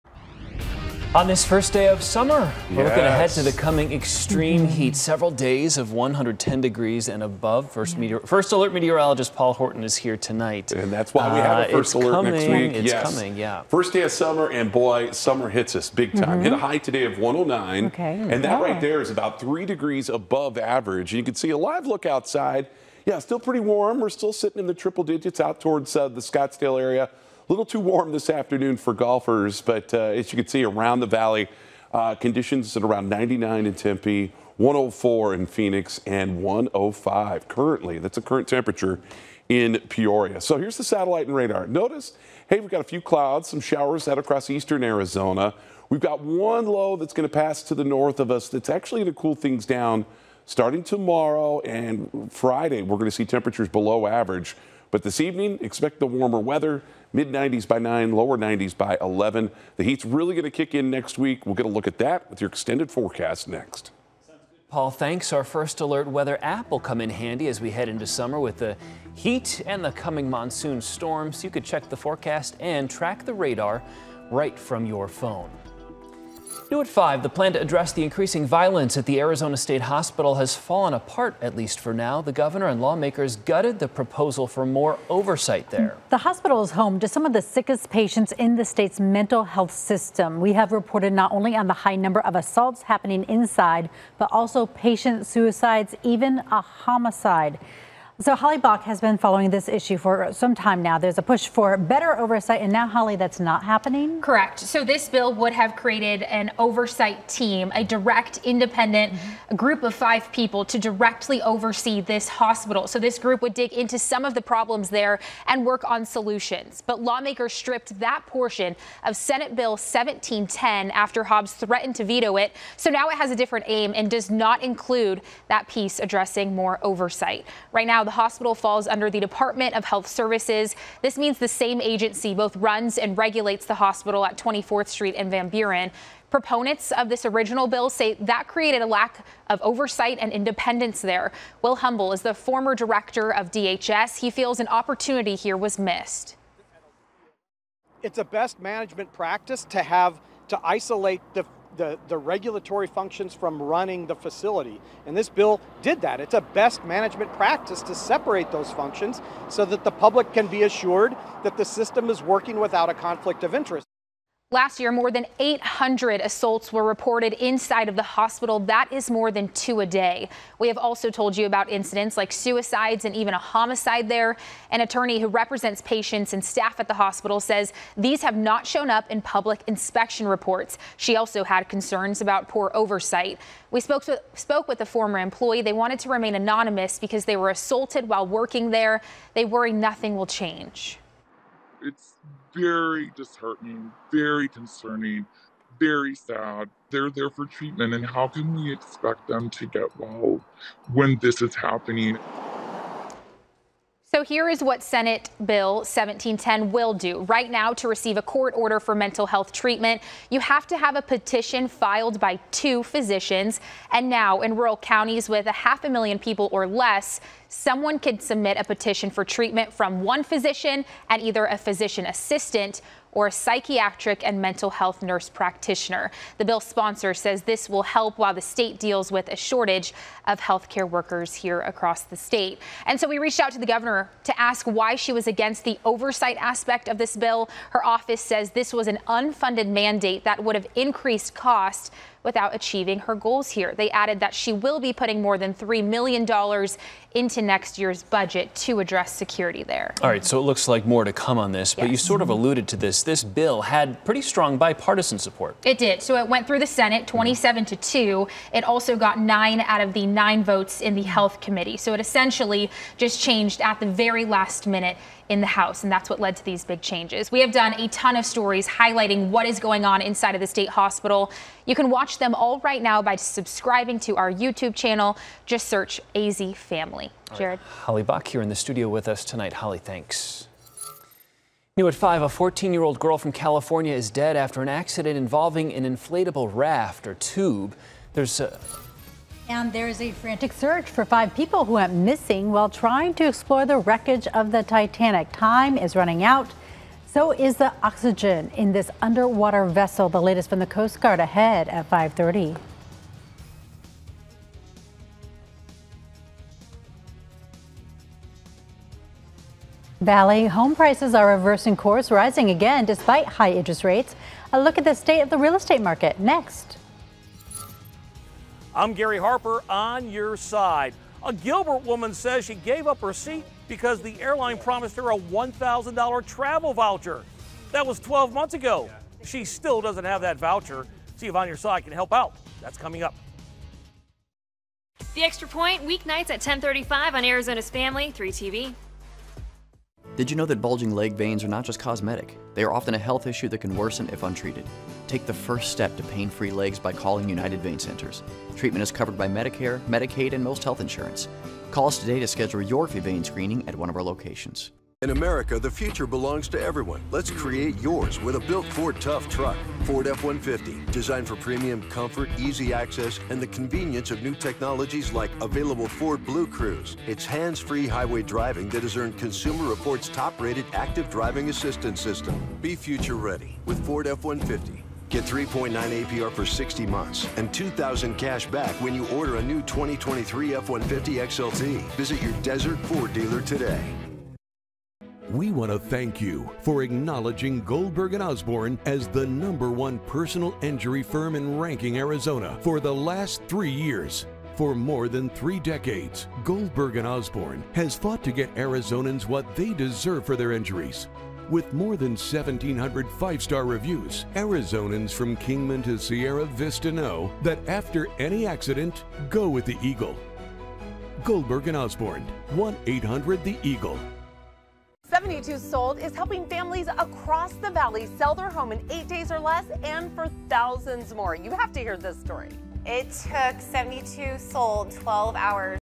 Test results offers insight into deadly bear attack south of Prescott
New test results released on Wednesday revealed new details about the bear who killed a Tucson man in Yavapai County.